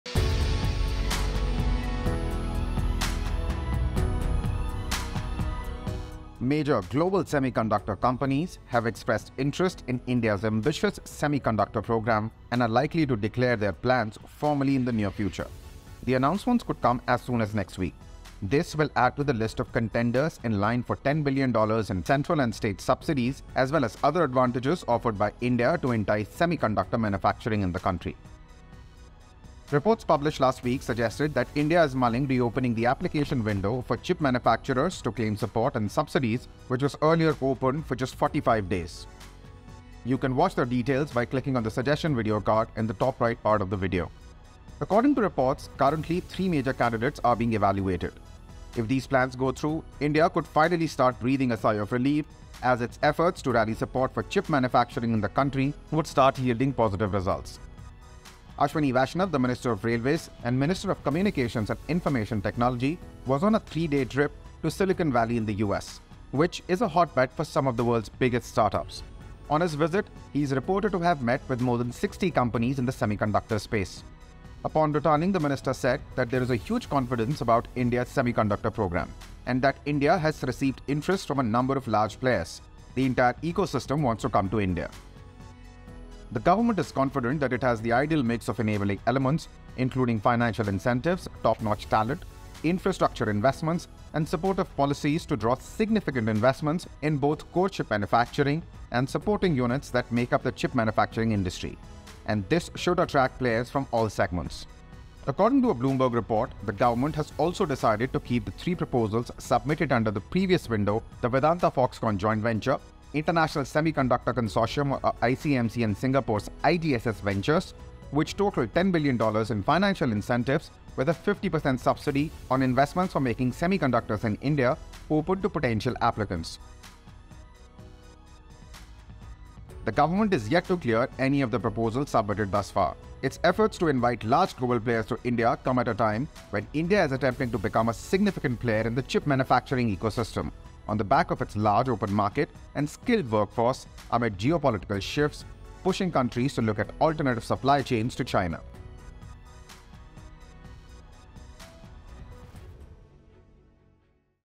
Chip Manufacturers Eye India Presence; India’s Efforts Yield Result | Mint In Focus
Major global semiconductor companies have expressed interest in India's ambitious semiconductor programme and are likely to declare their interest formally in the near future. The announcements could come as soon as next week. This will add to the list of contenders in line for $10 billion in central and state subsidies, as well as other advantages, offered by India to entice semiconductor manufacturing.

Reports published last week suggested that India is mulling reopening the application window for chip manufacturers to claim support and subsidies which was earlier limited to just 45 days at launch last year.